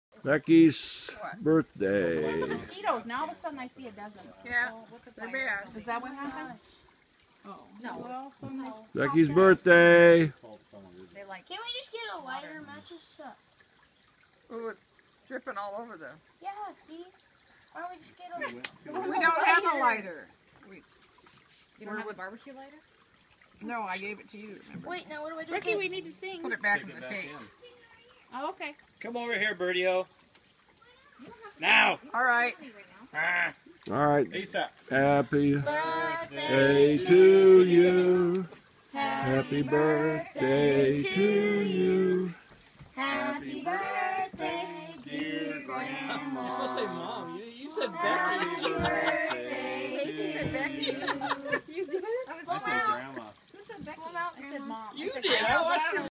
becky birthday video #17
Beckys 17th Birthday singing... and blowing out candles.